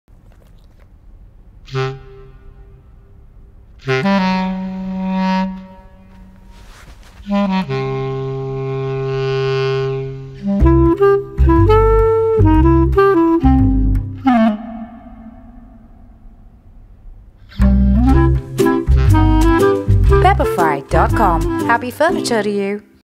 Pepperfry presents | Indoor Stories - 2
Spiders bugging you? Relax! We got it under control. Happy Furniture To You!